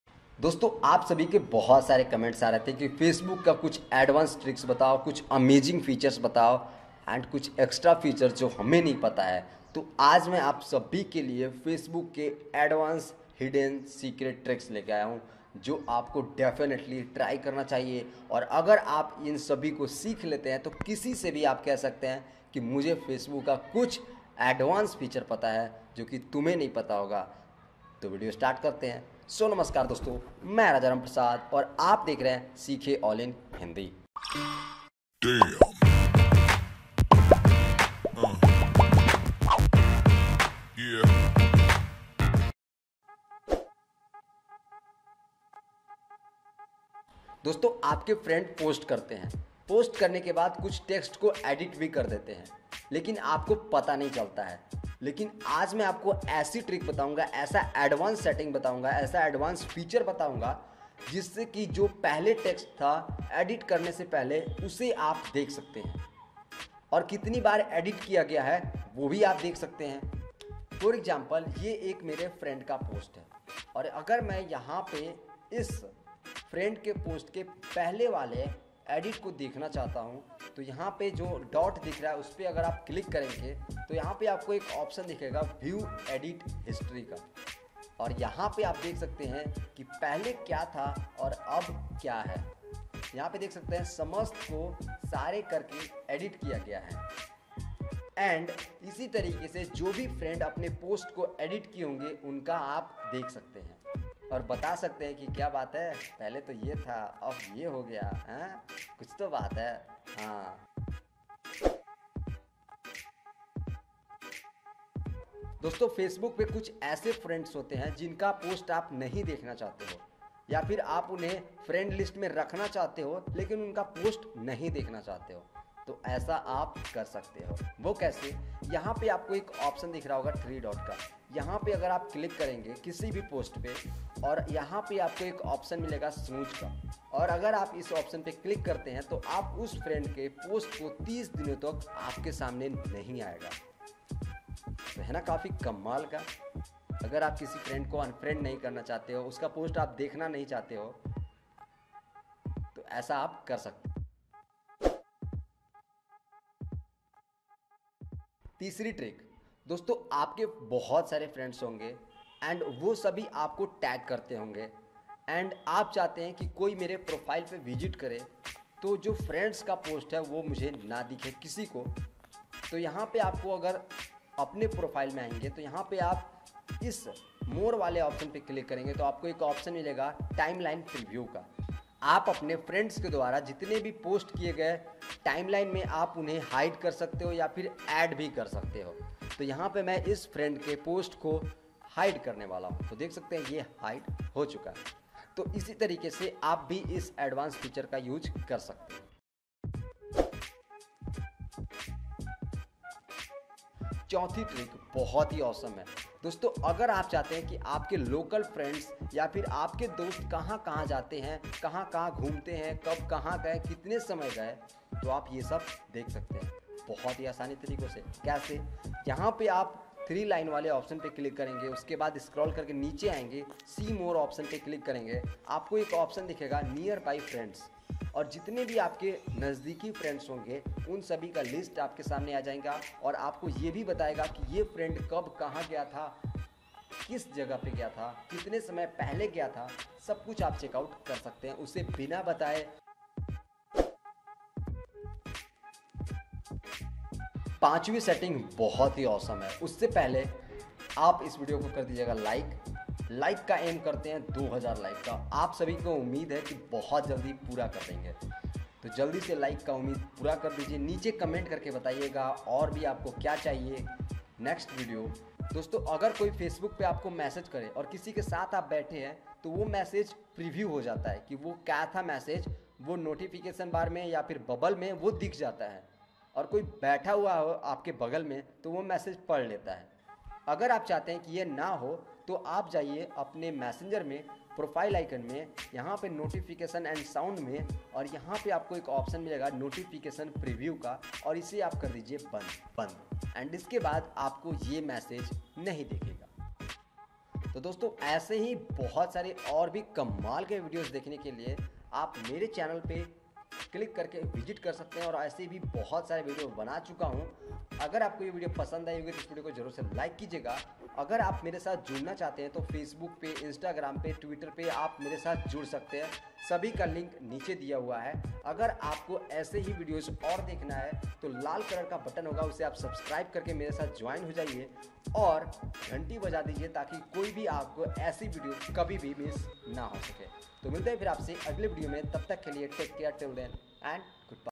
Facebook 5 Advance Tricks, Hidden Features, Secret Settings Change Now 2018
In this Video 5 new facebook tricks and tips jisse aap apne facebook ko advance users ki tarh use kar sakte hain agr aap in sabhi Facebook 5 Advance Tricks, Hidden Features, Secret Settings in Hindi 2018.